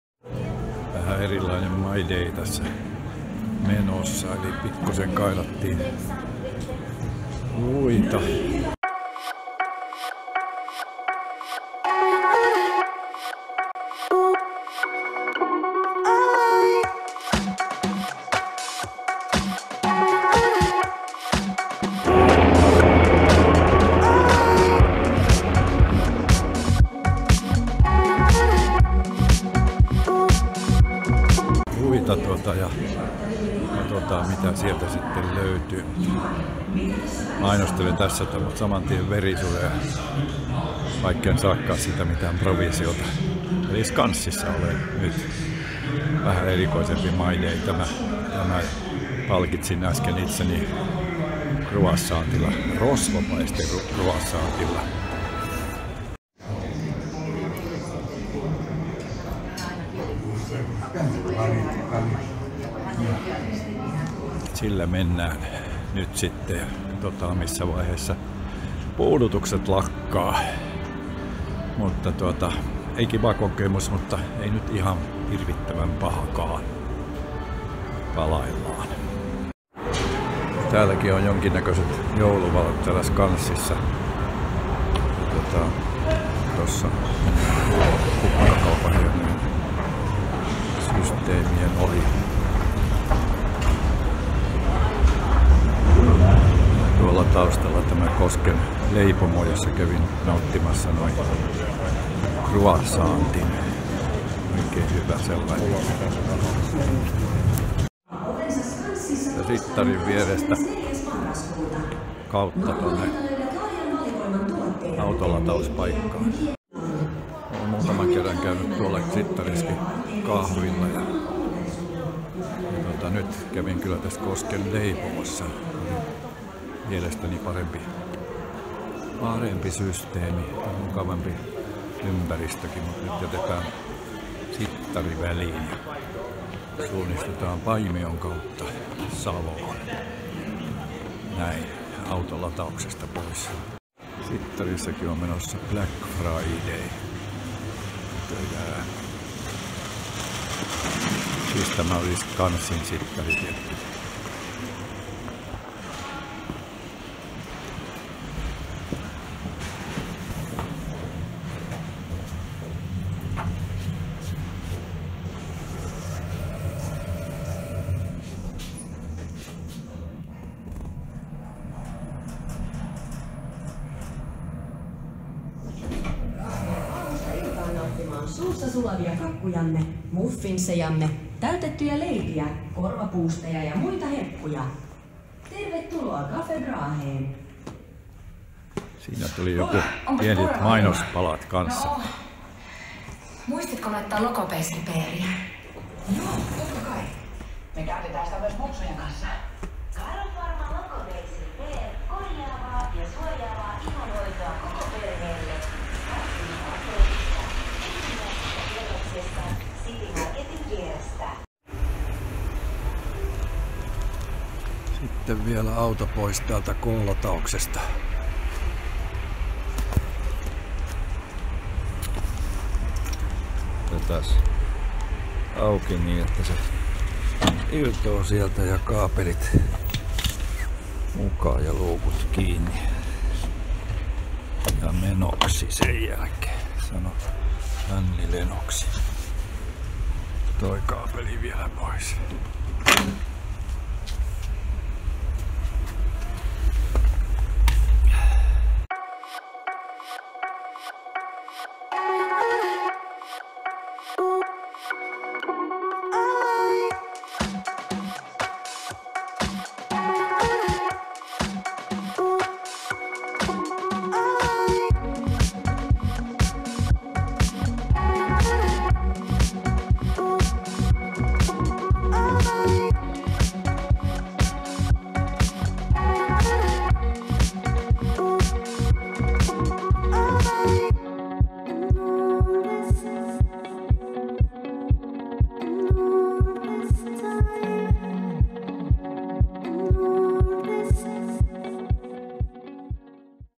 MY DAY. TYKS ja SKANSSI
Pieni  operaatio TYKS T-Sairaala ja sen perään Skanssiin "toipumaan" hetkeksi kahville.